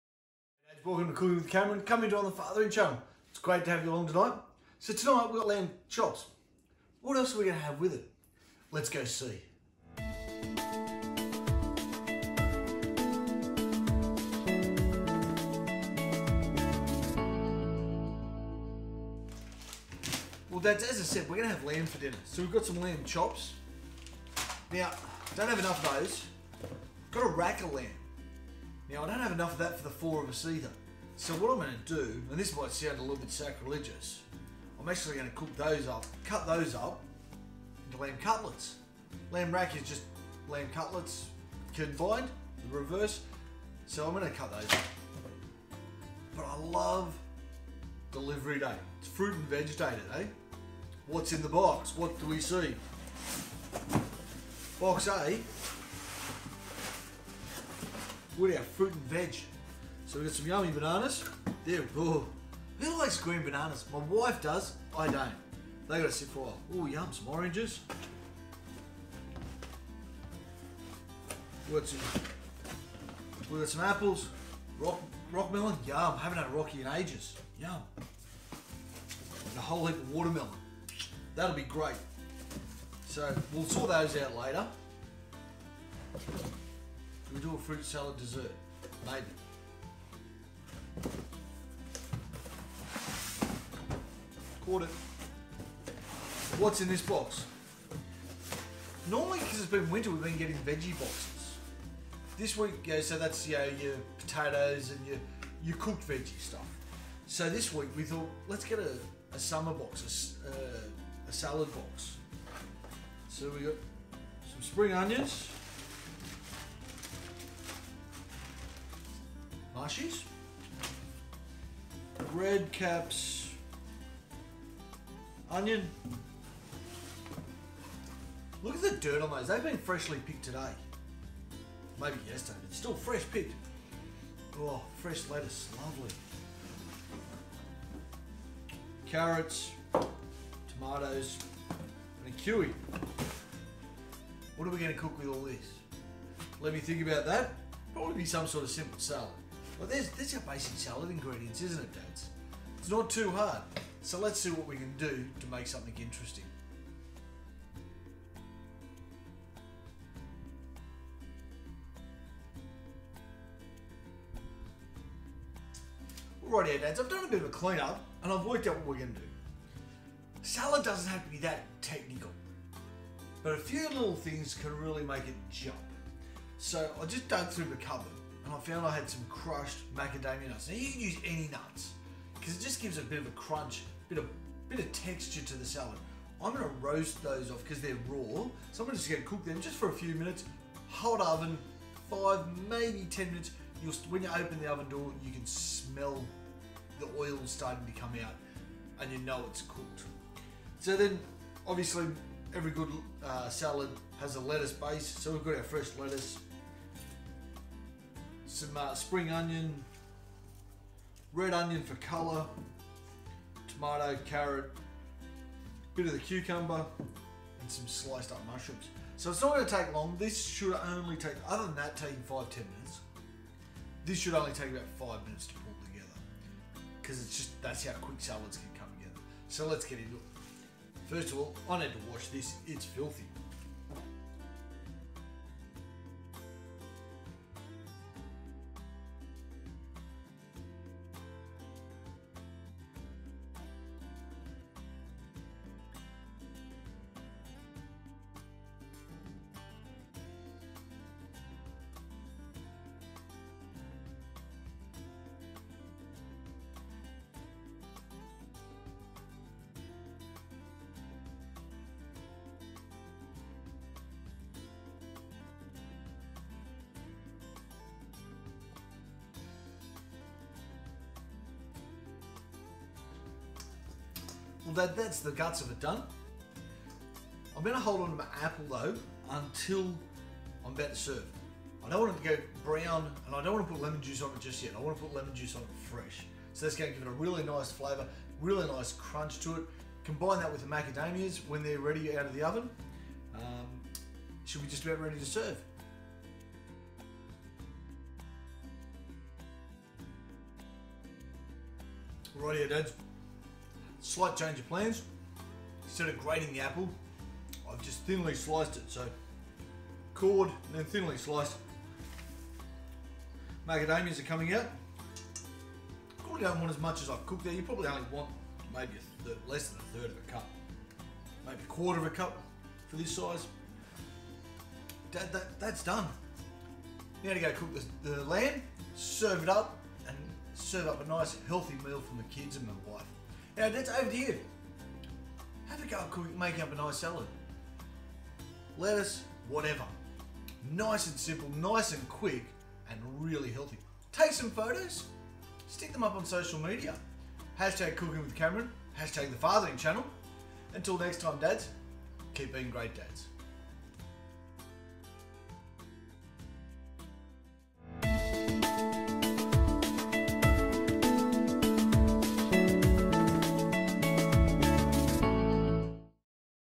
Cameron cooks up a perfect backyard BBQ for Boxing Day
Welcome back to cookingwithcameron on The Fathering Channel.

Cooking with Cameron –Boxing Day BBQ
Dads Apron up for a tasty Back Yard BBQ